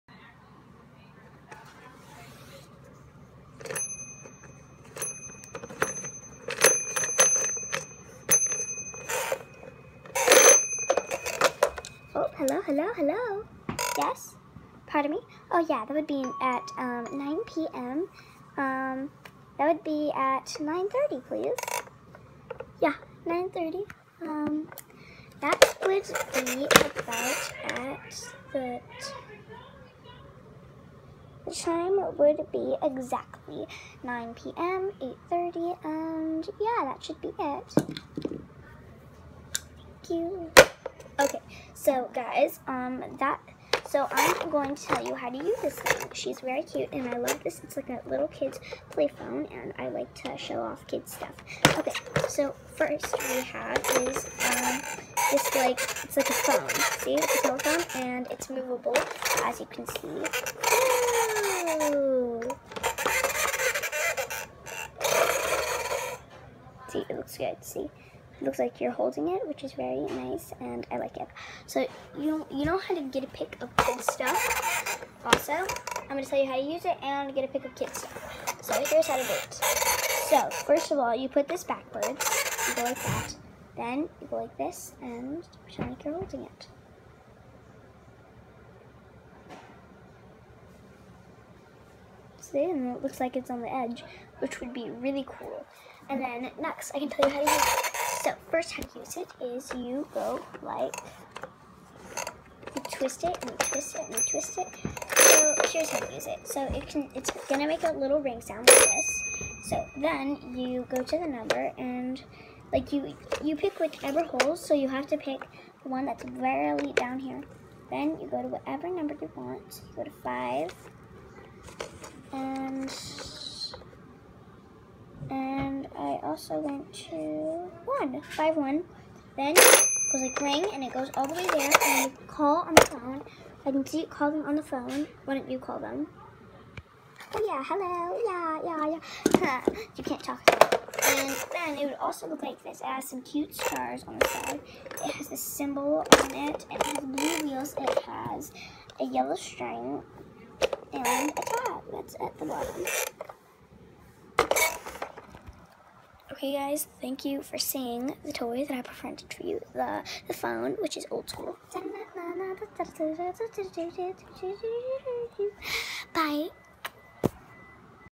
Jaxsen reviews chatterbox
A true review of the classic fisher price phone chatterbox
from a three year old.